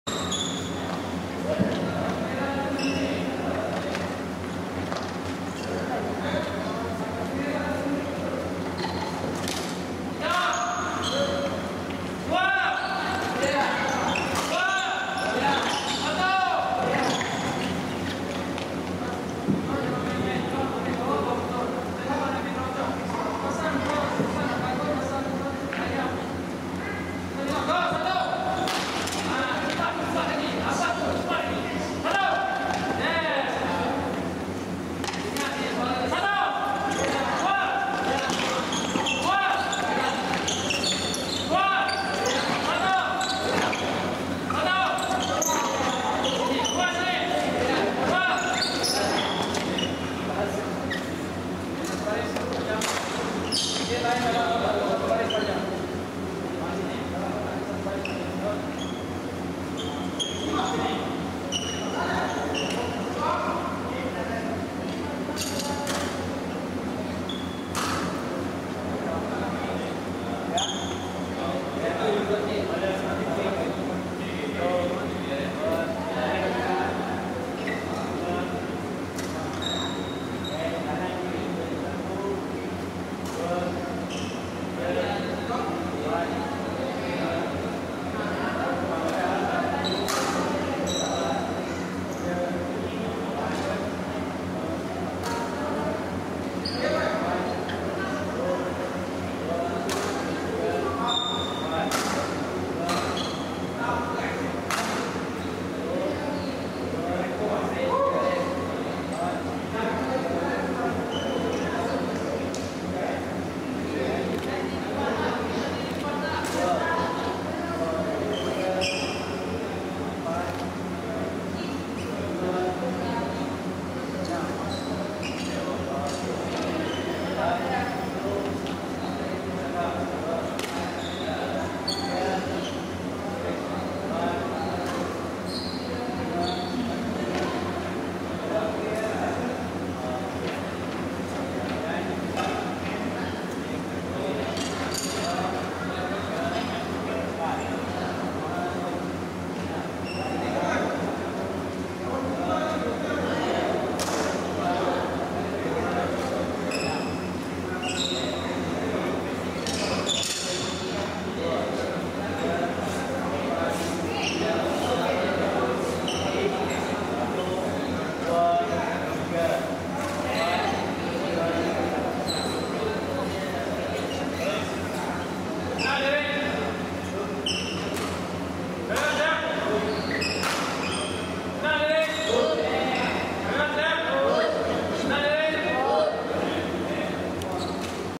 Program Khidmat Latihan Badminton Sukan Teras (KELAS) WPKL 2011 _ Sesi I
Program anjuran Jabatan Belia dan Sukan Wilayah Persekutuan Kuala Lumpur dengan kerjasama Nusa Mahsuri diadakan setiap hari Sabtu- Ahad bermula Oktober-November 2011. Ia disertai bakat-bakat muda berusia 9-12 tahun sekitar ibukota.